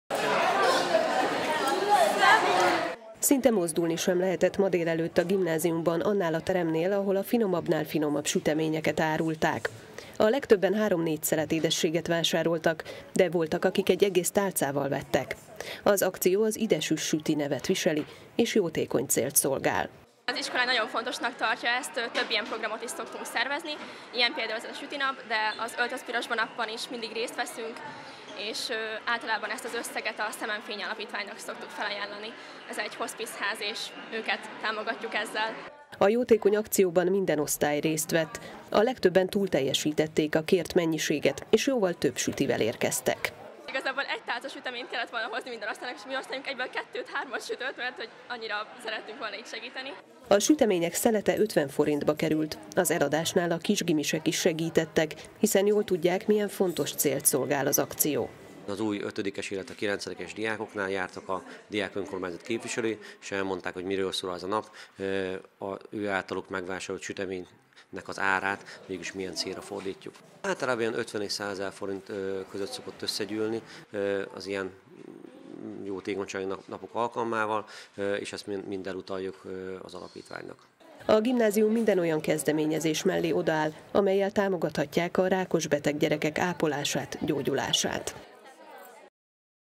Süteménnyel jótékonykodtak a gimisek
A mai nap a sütemények bűvöletében telt a Kossuth Gimnáziumban. Az iskola diákjai, illetve a szülők által sütött finomságokat lehetett megvásárolni a szünetekben. A befolyt összeget jótékony célokra fordították. Az akcióval a daganatos beteg gyerekeket ápoló intézményt támogatják.